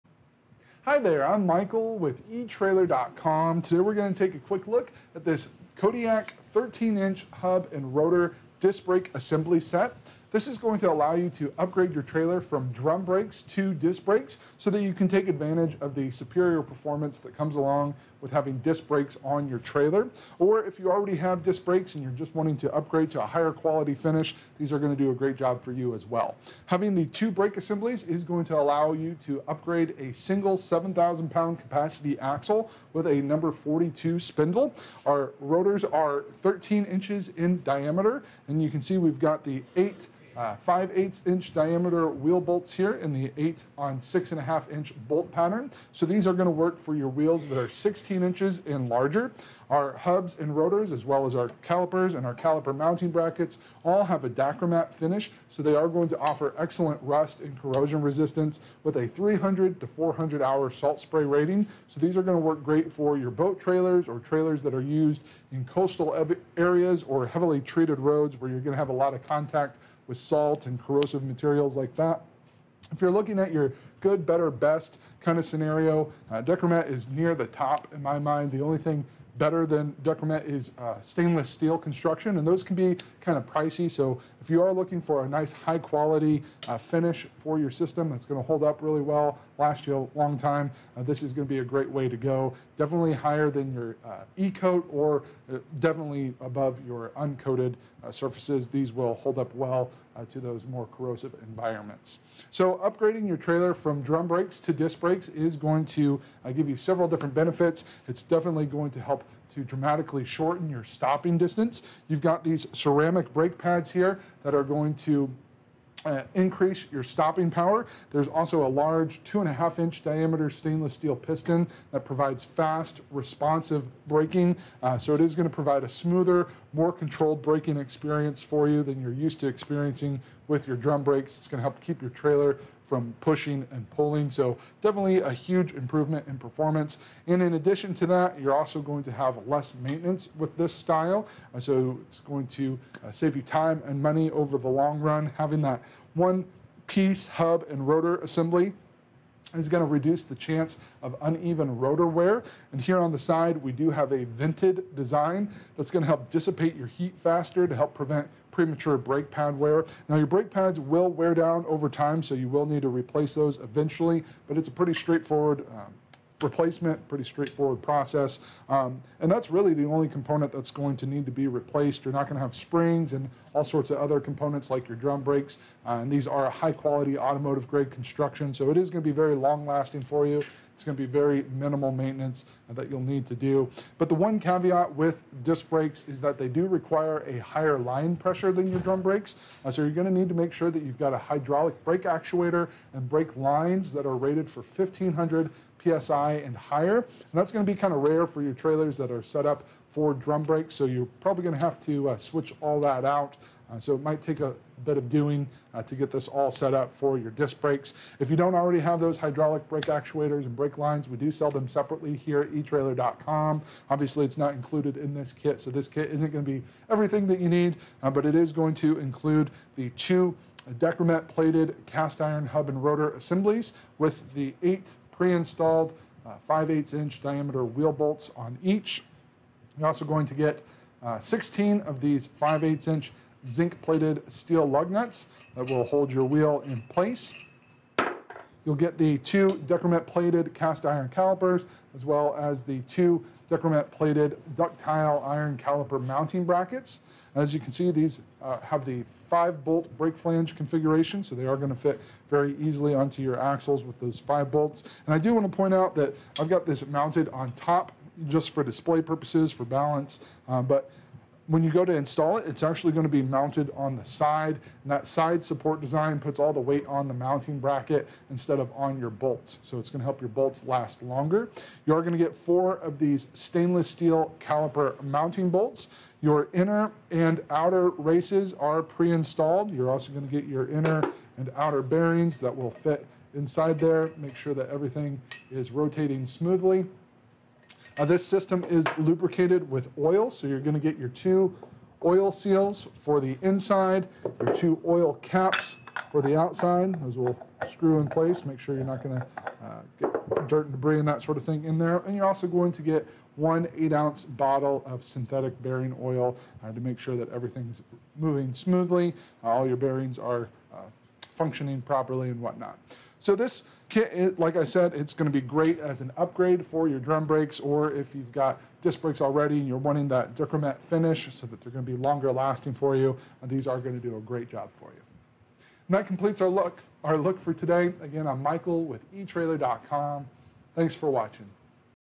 etrailer | Kodiak Disc Brakes-KOD73FR Spec Review
Hi there, I'm Michael with etrailer.com.  Today we're gonna take a quick look at this Kodiak 13" Hub and Rotor Disc Brake Assembly Set.  This is going to allow you to upgrade your trailer from drum brakes to disc brakes, so that you can take advantage of the superior performance that comes along with having disc brakes on your trailer.  Or, if you already have disc brakes and you're just wanting to upgrade to a higher quality finish, these are gonna do a great job for you as well.  Having the two brake assemblies is going to allow you to upgrade a single 7,000 pound capacity axle with a number 42 spindle.  Our rotors are 13" in diameter and you can see, we've got the 5/8" diameter wheel bolts here and the 8 on 6-1/2" bolt pattern.

So these are gonna work for your wheels that are 16" and larger.  Our hubs and rotors, as well as our calipers and our caliper mounting brackets, all have the Dacromet finish.  So they are going to offer excellent rust and corrosion resistance with a 300 to 400 hour salt spray rating.  So these are gonna work great for your boat trailers, or trailers that are used in coastal areas, or heavily treated roads, where you're gonna have a lot of contact with salt and corrosive materials like that.  If you're looking at your good, better, best, kind of scenario, Dacromet is near the top in my mind.

The only thing better than Dacromet is stainless steel construction, and those can be kind of pricey so if you are looking for a nice, high-quality finish for your system, it's gonna hold up really well, last you a long time, this is gonna be a great way to go.  Definitely higher than your E-coat, or definitely above your uncoated surfaces.  These will hold up well to those more corrosive environments.  So upgrading your trailer from drum brakes to disc brakes is going to give you several different benefits.  It's definitely going to help to dramatically shorten your stopping distance.

You've got these ceramic brake pads here that are going to increase your stopping power.  There's also a large 2-1/2" diameter stainless steel piston that provides fast, responsive braking.  So it is gonna provide a smoother, more controlled braking experience for you than you're used to experiencing with your drum brakes.  It's gonna help keep your trailer from pushing and pulling, so definitely a huge improvement in performance.  And in addition to that, you're also going to have less maintenance with this style, so it's going to save you time and money over the long run.

Having that one-piece hub-and-rotor assembly is gonna reduce the chance of uneven rotor wear.  And here on the side, we do have a vented design.  That's gonna help dissipate your heat faster, to help prevent premature brake pad wear.  Now, your brake pads will wear down over time so you will need to replace those eventually, but it's a pretty straightforward replacement, pretty straightforward process, and that's really the only component that's going to need to be replaced.  You're not gonna have springs and all sorts of other components like your drum brakes.  And these are a high-quality automotive grade construction, so it is gonna be very long-lasting for you.  It's gonna be very minimal maintenance that you'll need to do.  But the one caveat with disc brakes is that they do require a higher line pressure than your drum brakes.  So you're gonna need to make sure that you've got a hydraulic brake actuator and brake lines that are rated for 1500 PSI and higher.  And that's gonna be kind of rare for your trailers that are set up for drum brakes, so you're probably gonna have to switch all that out, so it might take a bit of doing to get this all set up for your disc brakes.  If you don't already have those hydraulic brake actuators and brake lines, we do sell them separately here at etrailer.com.  Obviously, it's not included in this kit.  So this kit isn't gonna be everything that you need but it is going to include the two Dacromet-plated, cast iron hub-and-rotor assemblies with the eight pre-installed 5/8" diameter wheel bolts on each.  You're also going to get 16 of these 5/8" zinc-plated steel lug nuts that will hold your wheel in place.  You'll get the two Dacromet-plated cast iron calipers, as well as the two Dacromet-plated ductile iron caliper mounting brackets.  As you can see, these have the five bolt brake flange configuration so they are gonna fit very easily onto your axles with those five bolts.  And I do wanna point out that I've got this mounted on top just for